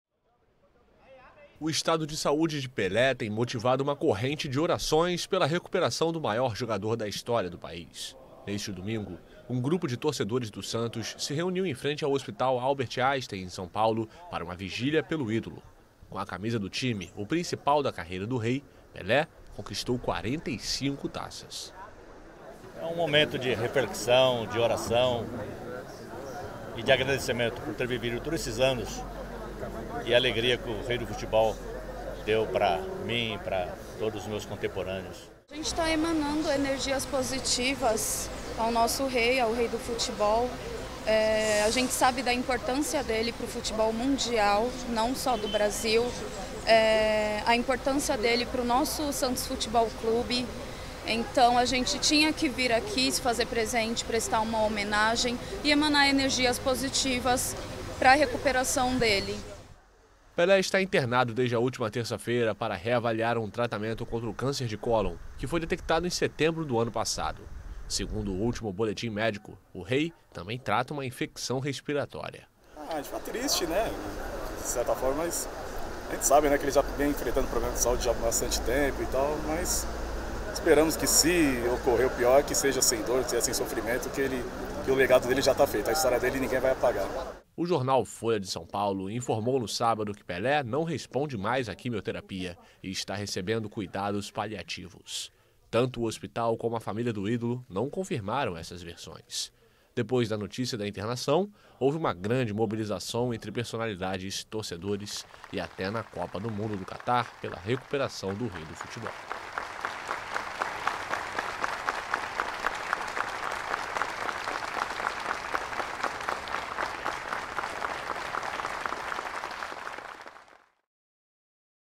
Torcedores do Santos fazem vigília por Pelé | AFP
Um grupo de mais de 100 torcedores do Santos se reuniu, neste domingo (4), em frente ao hospital no qual Pelé está internado em São Paulo para rezar pela recuperação do ídolo e mandar uma mensagem de apoio.